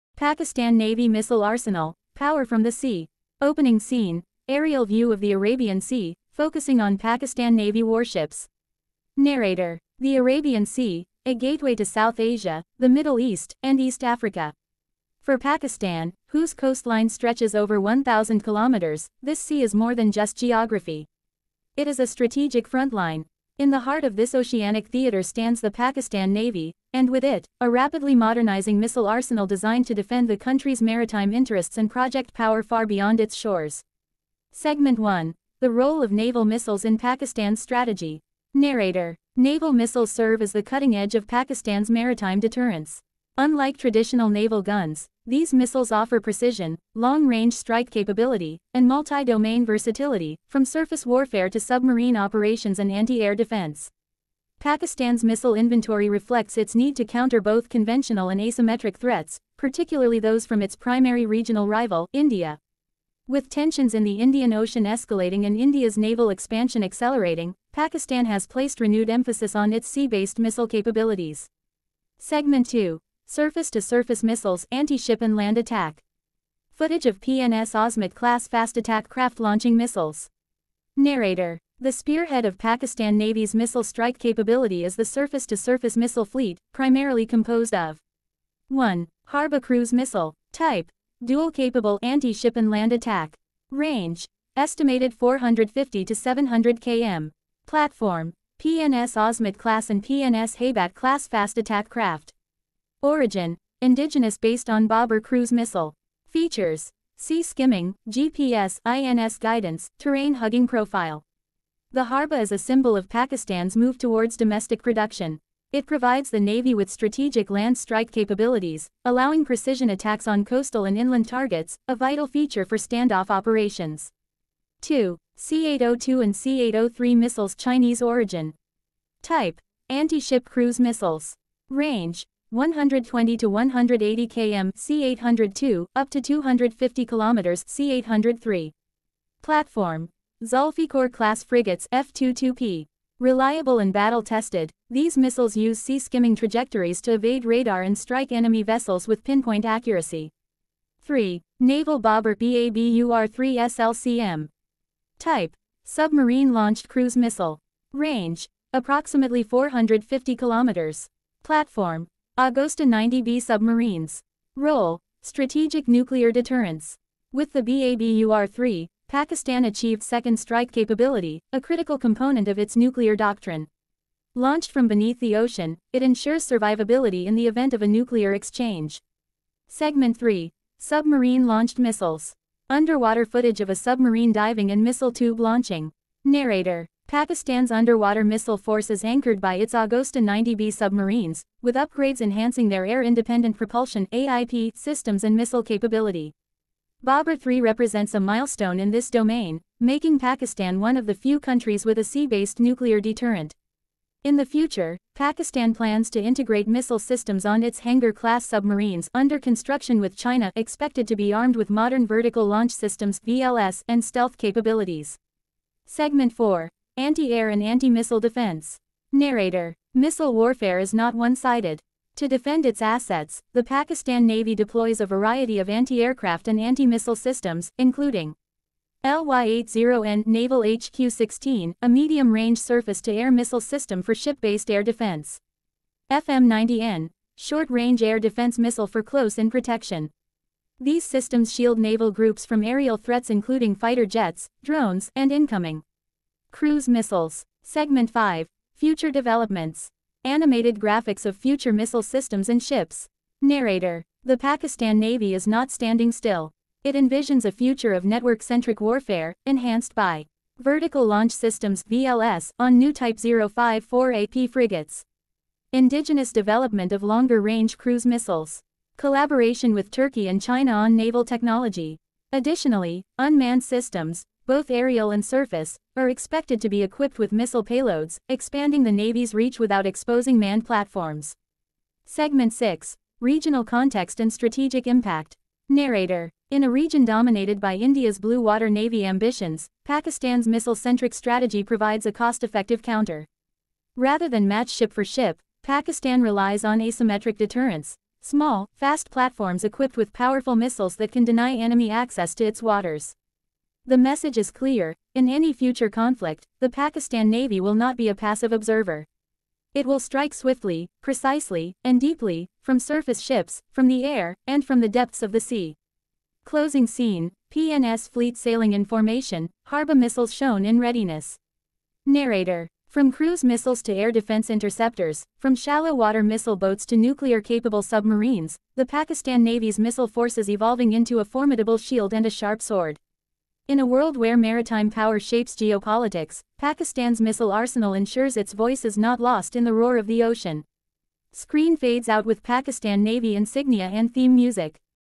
"Pakistan Navy Missiles 2025 – Full Explained | Harbah, Babur-3, Anti-Ship & Submarine Missiles"
Discover the full power of the Pakistan Navy’s missile arsenal in this detailed documentary. From the Harbah cruise missile to the Babur-3 submarine-launched missile, this video explains every major naval weapon system in Pakistan's maritime force. Learn about surface-to-surface missiles, anti-ship systems, submarine-launched cruise missiles, and future developments. Watch how Pakistan maintains naval superiority in the Arabian Sea and counters threats with high-tech precision weaponry. Updated for 2025, this is your complete guide to Pakistan’s sea-based missile strategy

Pakistan Navy missiles, Harbah missile, Babur-3 missile, Pakistan cruise missiles, submarine-launched missile, Pakistan Navy missile 2025, Pakistan naval weapons, anti-ship missile